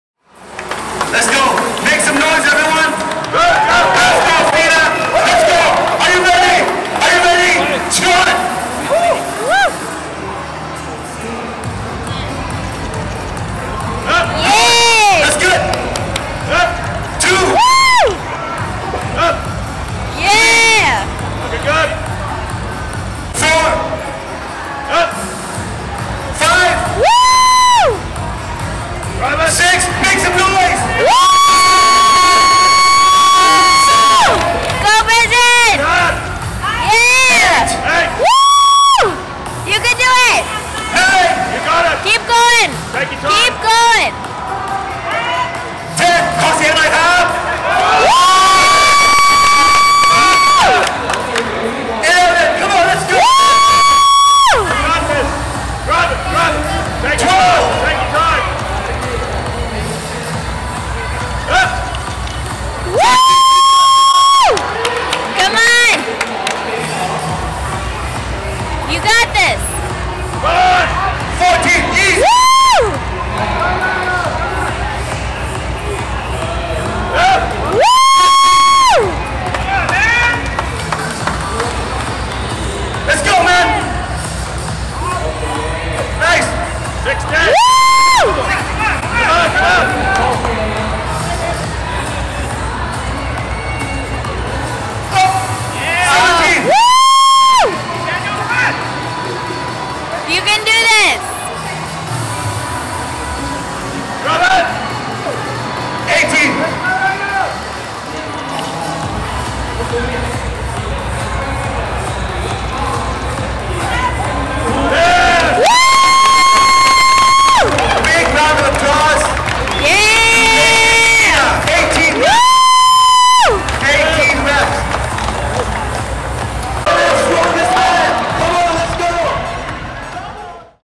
Strongman Competition 2 time Body weight for max Squat | FitFest Strongman Competition 2017
Strongman Competition 2-time Bodyweight for max Squat
Well, this was a new PR (personal record) for me - 2 times bodyweight for 18 reps! It was not easy since I was off for 6 months without any leg work out. Within the past 2 months, I have been slowly coming back stronger. In the past, I would have hit this for 30 reps.